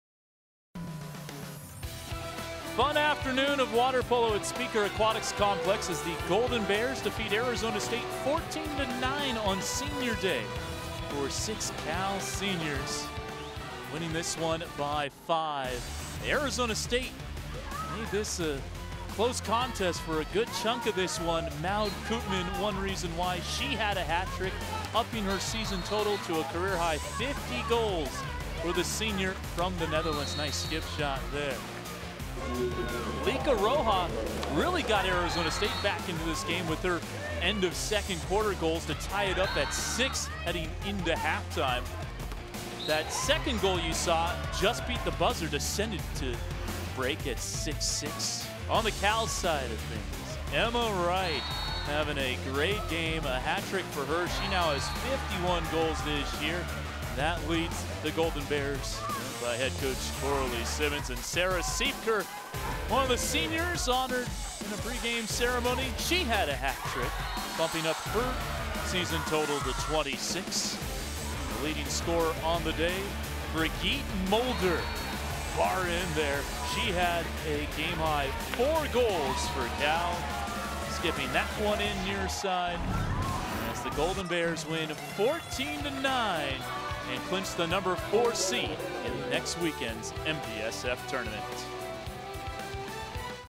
Recap: No. 4 Cal women's water polo racks up three hat tricks in win over Arizona State
Pac-12 Networks' Kevin Danna looks back at Cal's 14-9 victory over Arizona State at Spieker Aquatics Complex at Senior Day on Saturday. The Bears tallied three hat tricks over the No. 10 Sun Devils in the regular-season finale. For the Bears, sophomore Brigit Mulder finished with four goals for the while senior Sarah Siepker and junior Emma Wright added three. Goalie Madison Tagg also made nine saves for Cal.